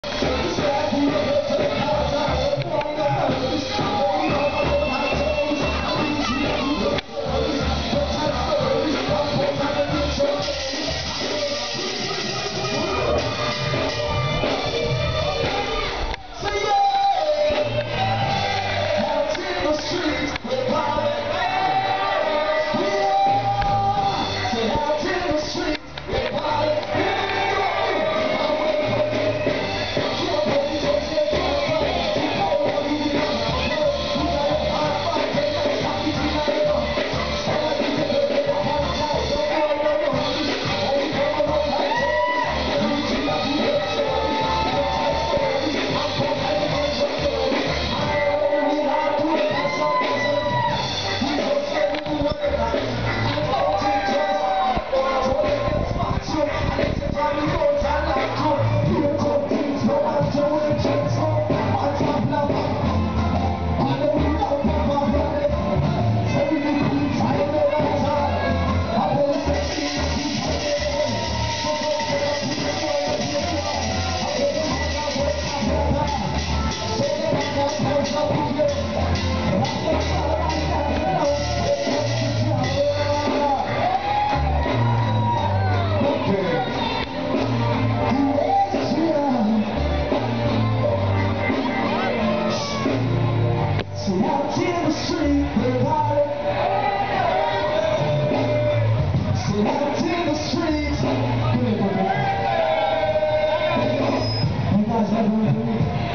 Mangos Miami South Beach
Mangos in Miami. One of the fun spots to hang out. Their signature mojitos are delicious! Very entertaining spot. Can't wait to go back!